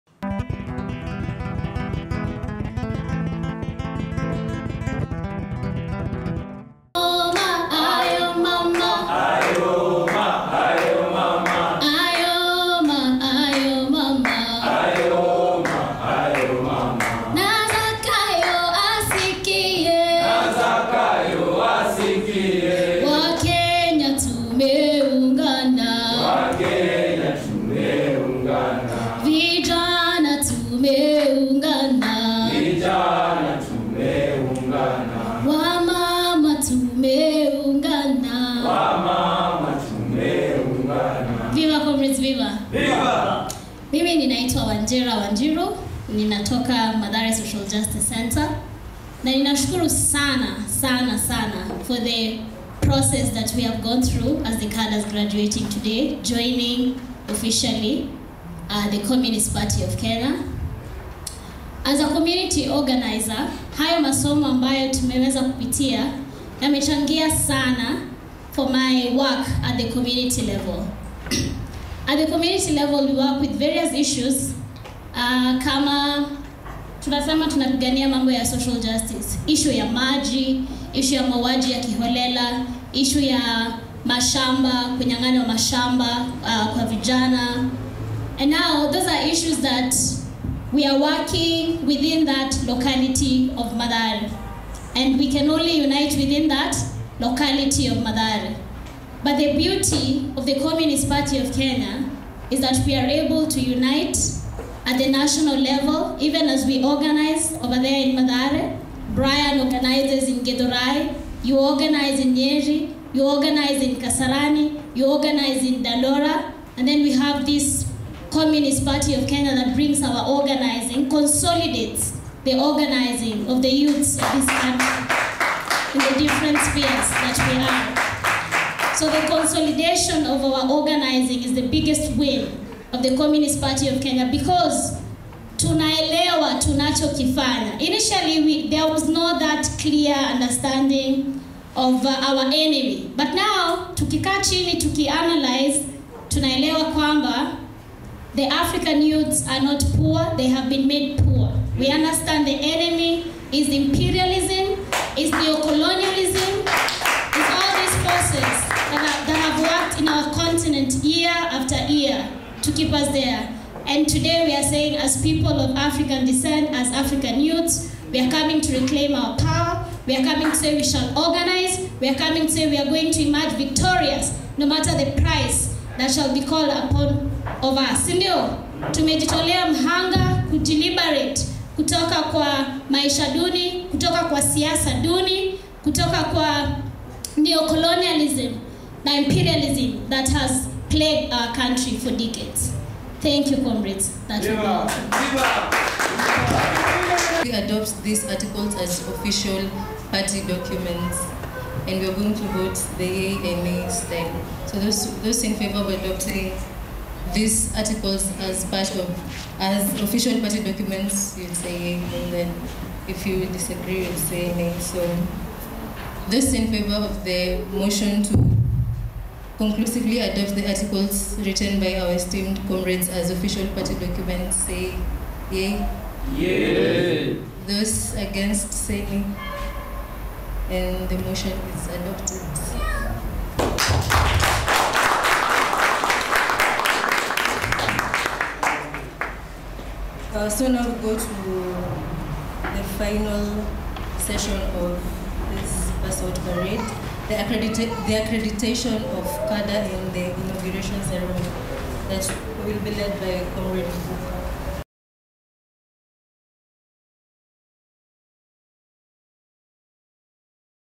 Ideological document reflection ( Cadre 4)
Communist Party of Kenya is founded on Marxist-Leninist ideology which require its members and cadres to involve in proletariat political education, Cadres are therefore required to have  collective documents discussion and reflection during party meetings.